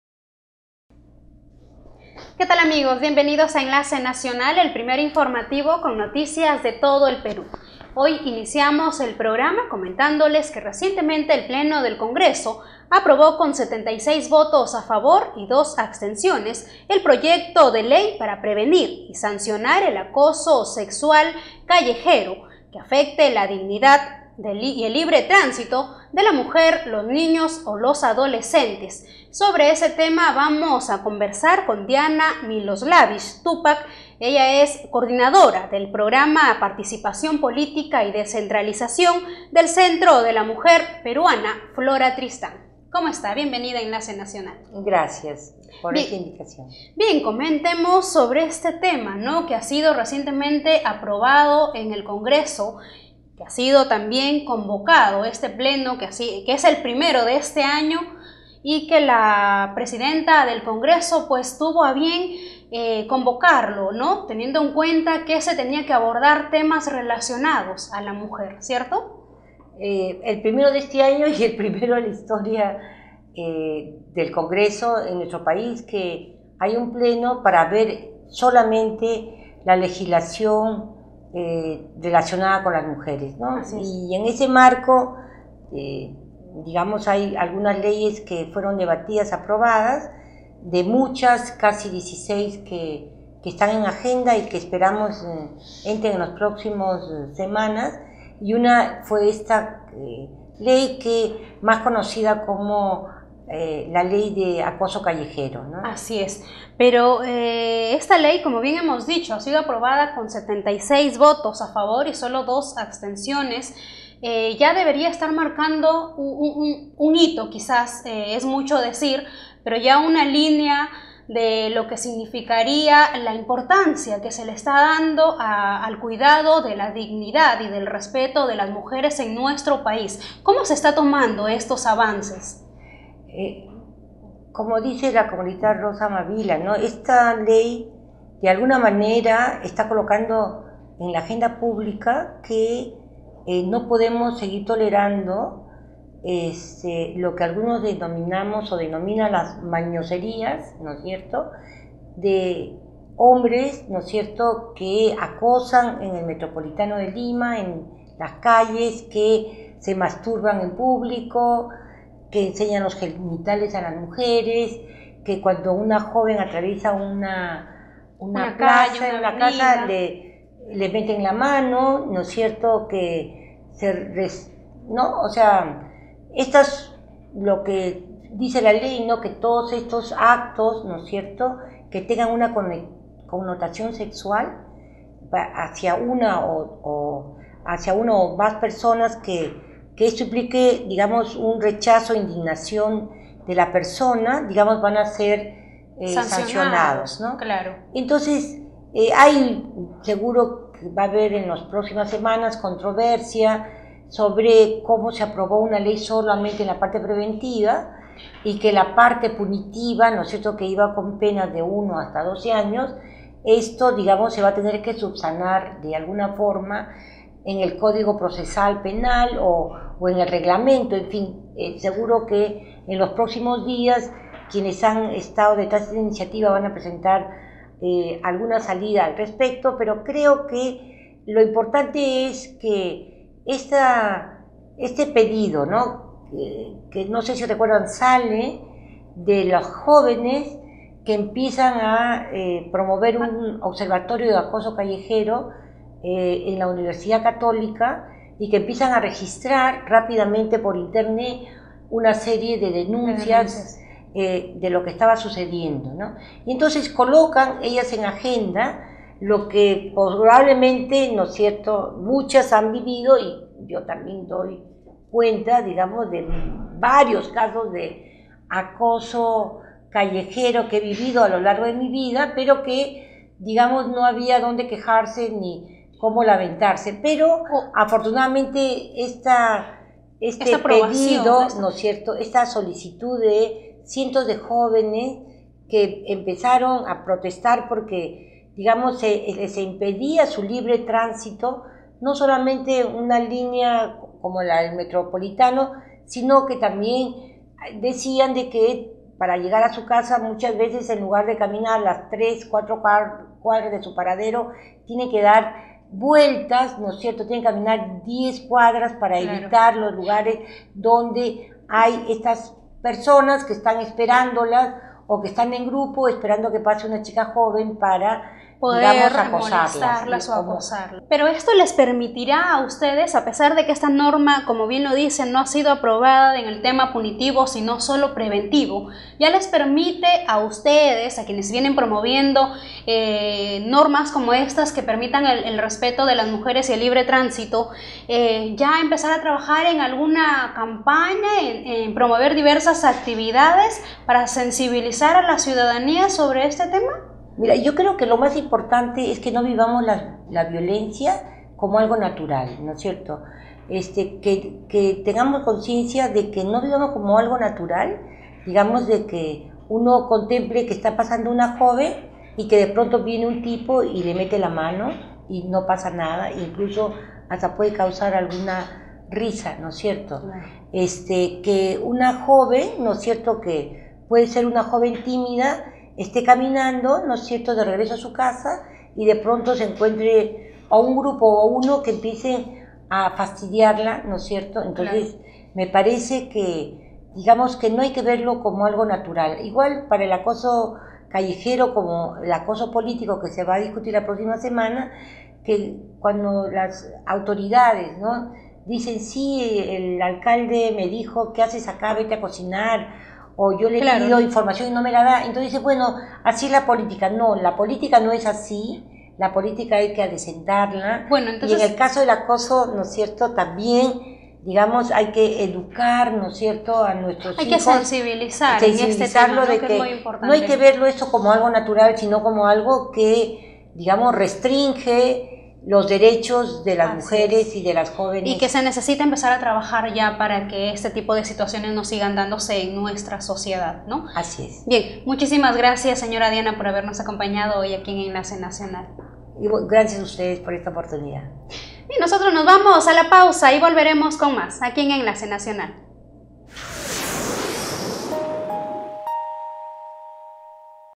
Diana Miloslavich: Ley contra el acoso sexual callejero marca un hito en la sociedad
El miércoles 5 de febrero fue aprobado por el pleno del Congreso, con 76 votos a favor y dos abstenciones, el proyecto de ley para Prevenir y Sancionar el Acoso Sexual en los Espacios Públicos que afecten la dignidad, la libertad, el libre tránsito y el derecho a la integridad física y moral de niños, adolescentes y mujeres. Al respecto, conversamos con Diana Miloslavich, coordinadora del Programa de Participación Política y Descentralización del Centro de la Mujer Peruana Flora Tristán.
"No podemos seguir tolerando lo que muchas personas denominan 'mañoserías' en los diversos espacios públicos", expresó Miloslavich.
La especialista de Flora Tristán explicó que esta norma ha sido aprobada de manera preventiva, mientras que los aspectos punitivos quedan pendientes para ser subsanados en el Nuevo Código Procesal Penal (NCPP) o en el reglamento.
"Esto tiene que ver con que todos los seres humanos seamos hombres o mujeres tengamos la libertad de transitar por las calles sin que nadie viole nuestro libre tránsito", enfatizó la coordinadora de Flora Tristán.